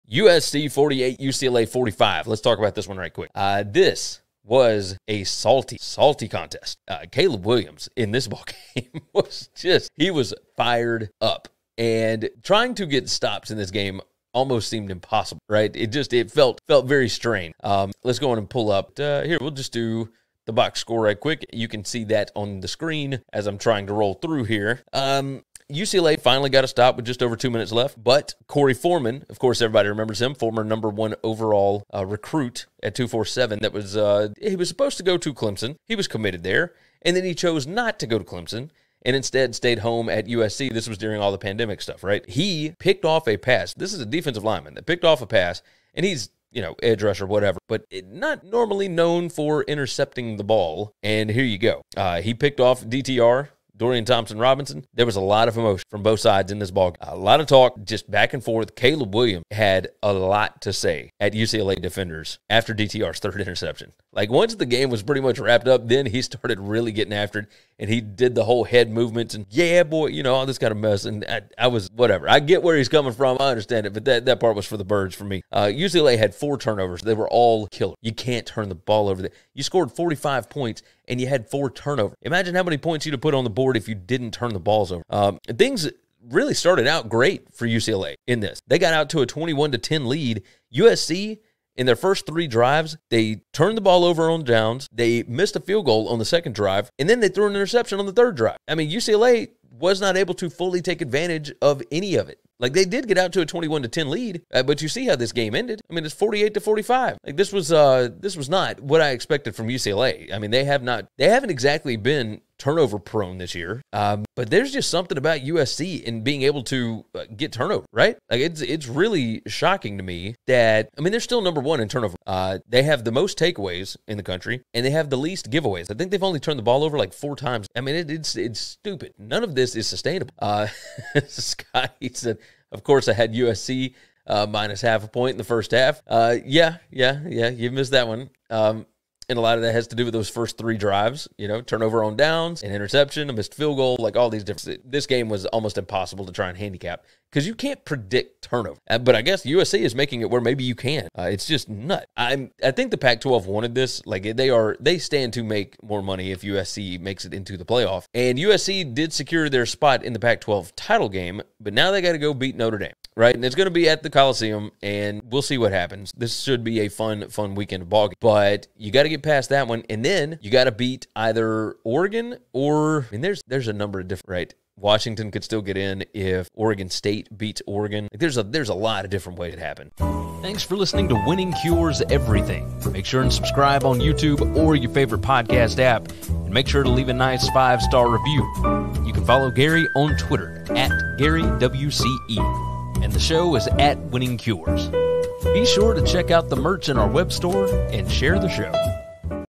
USC vs UCLA quick reaction & recap 2022 College Football
Gary gives his quick reaction, breakdown, analysis & recap to USC’s big win vs UCLA in Week 12 of the 2022 College Football season.  What do we make of USC coach Lincoln Riley or UCLA coach Chip Kelly after this one?  What’s the story, what happened here, what do the stats say, etc?